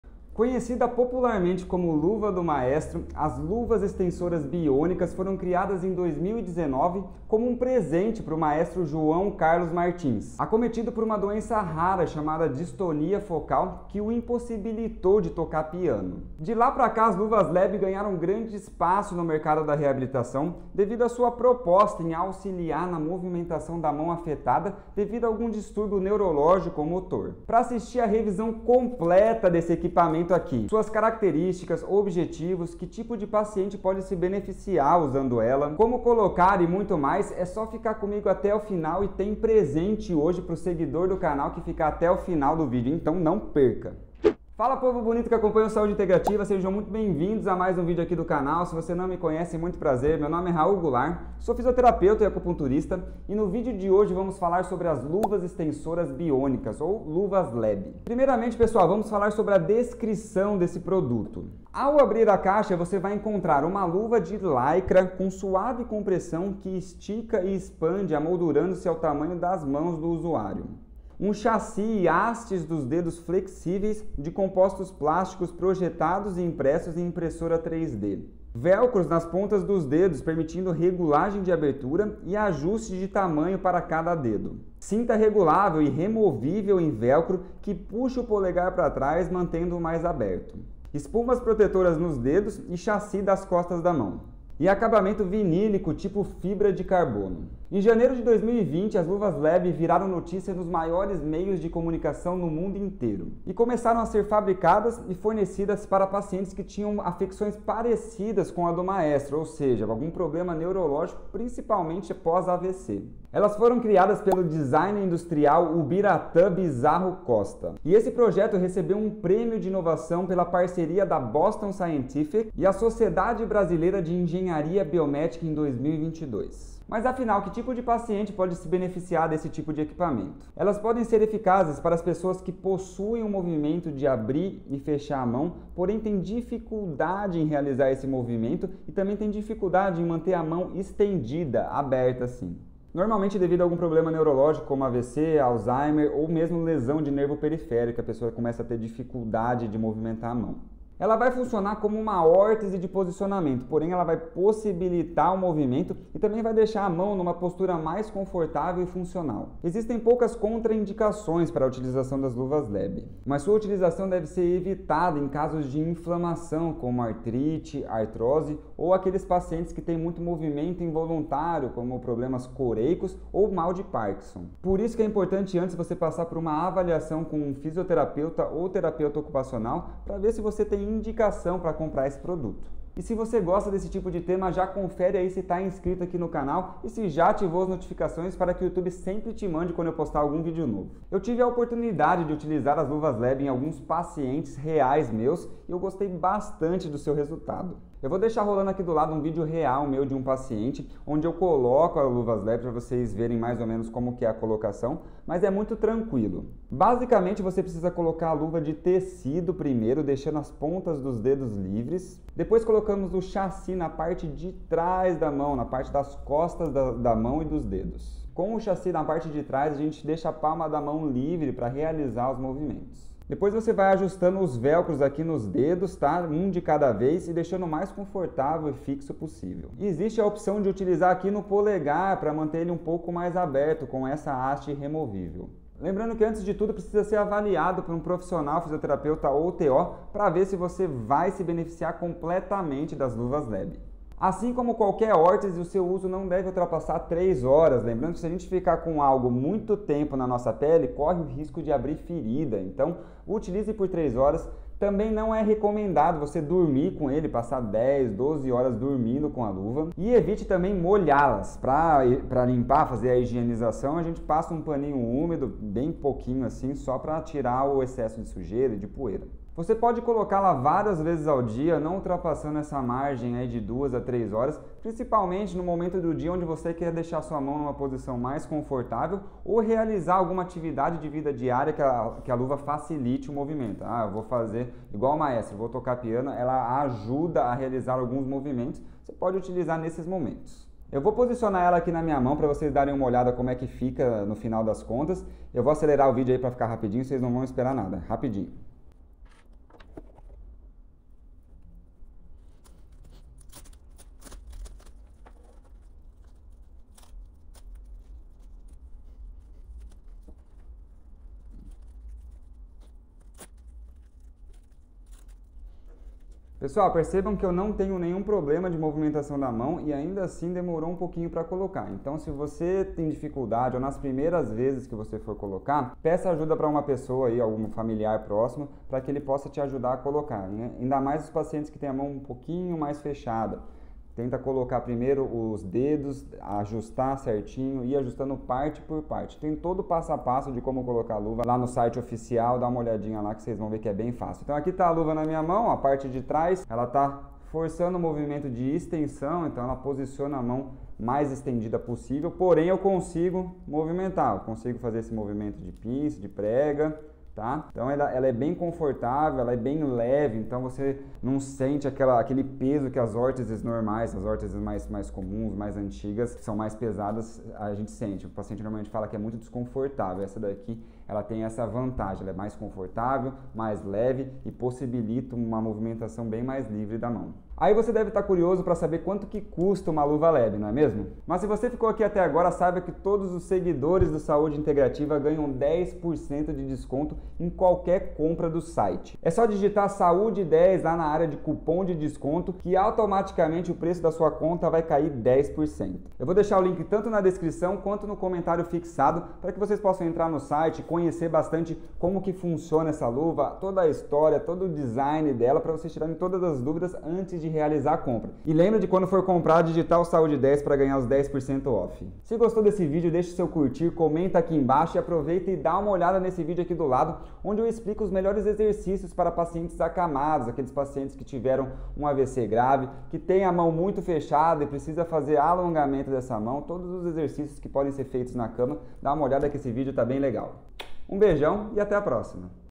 COMO FUNCIONAM AS LUVAS EXTENSORAS BIÔNICAS? - Review completo das Luvas LEB/Luva do Maestro.
No vídeo de hoje eu apresento o review completo das Luvas LEB, popularmente conhecidas como Luva do Maestro. Suas principais características, como funcionam, como colar e tudo o que você precisa saber sobre as Luvas Extensoras Biônicas estão contidas nesse vídeo.

Produtos de Fisioterapia e Acupuntura